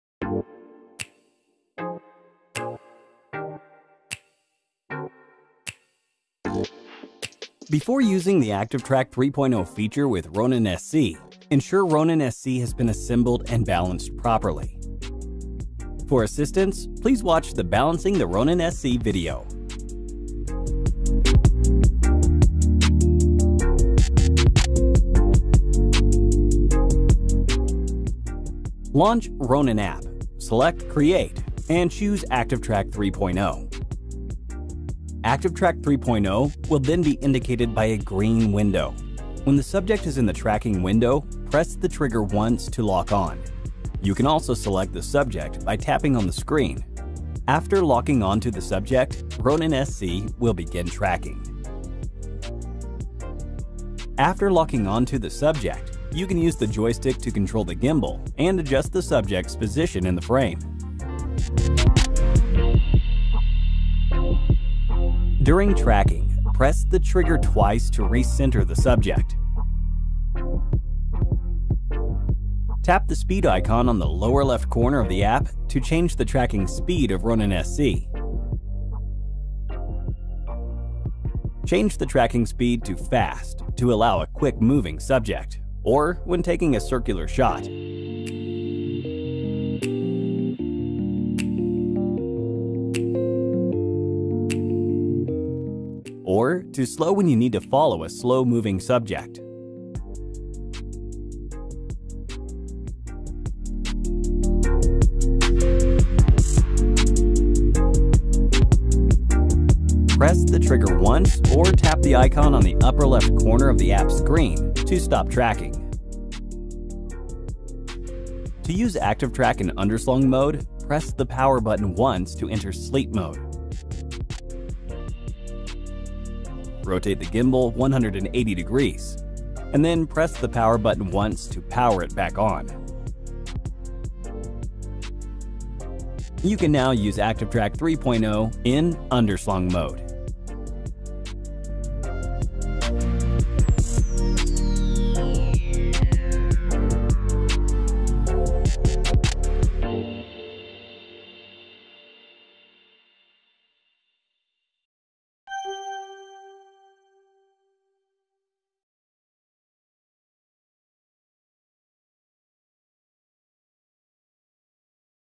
Ronin-SC - Jak korzystać z funkcji Activetrack 3.0 (PL)
W dzisiejszym poradniku pokażemy Wam jak korzystać z naszej najbardziej zaawansowanej funkcji śledzenia Activetrack 3.0.